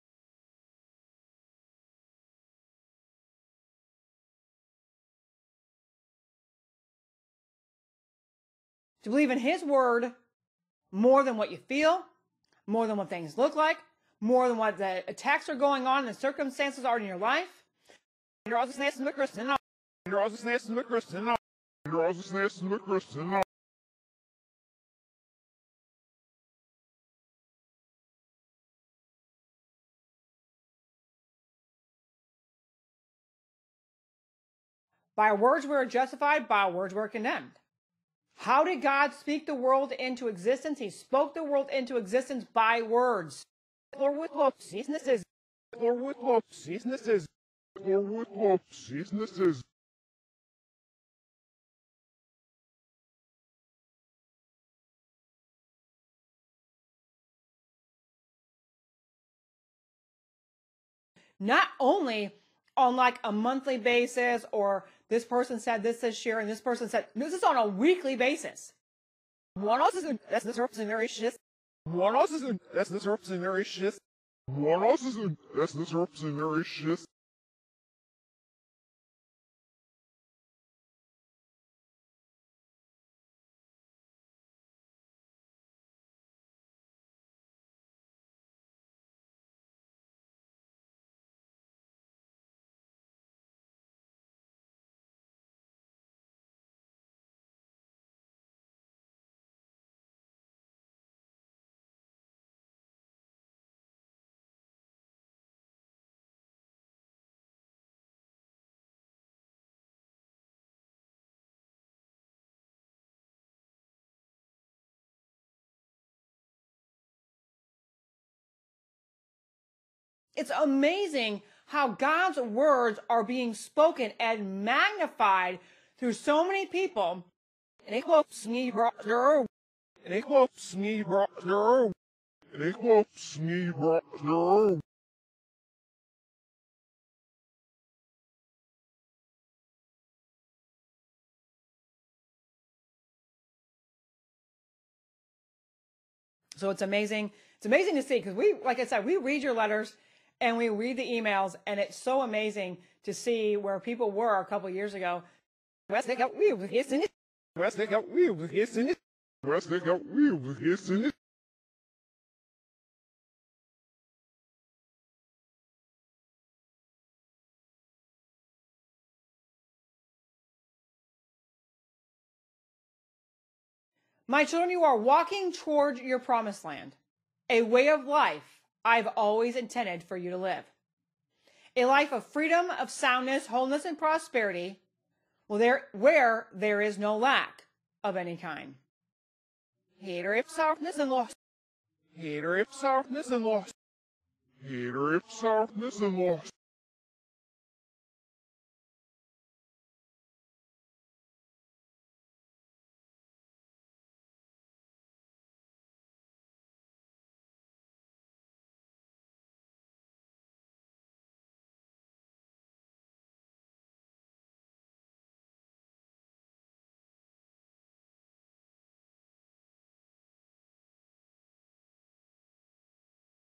Reverse Speech analysis of Julie Green, Julie Green Ministries
Reverse Speech analysis of Julie Green, Julie Green Ministries prepared by David John Oates - it gives me no joy posting these - the truth is tough